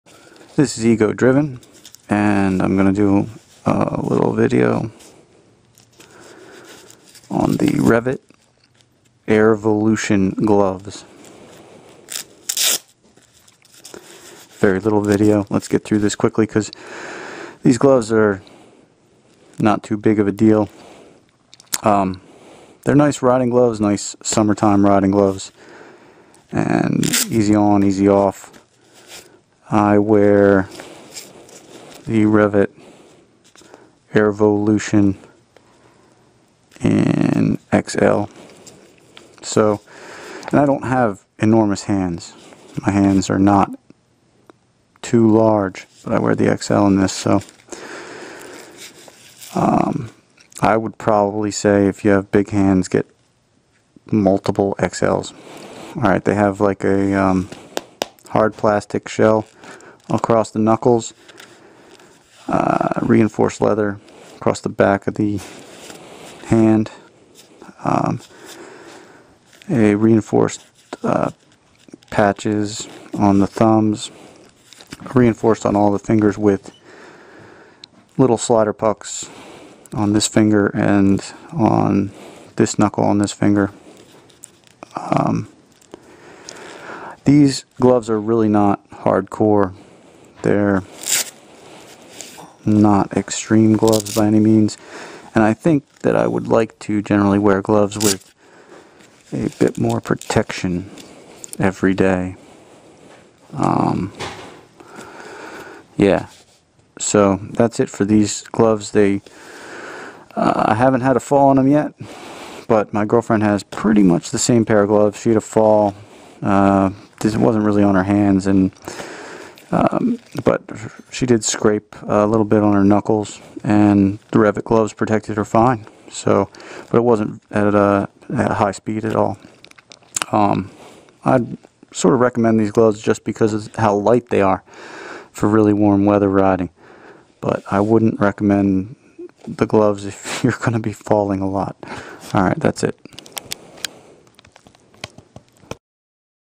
Gear Review, Rev-It Airvolution
Review of the Rev-It Airvolution gloves. Short video featuring the aforementioned gloves, nothing special just thought you might want to take a closer look at them before you buy.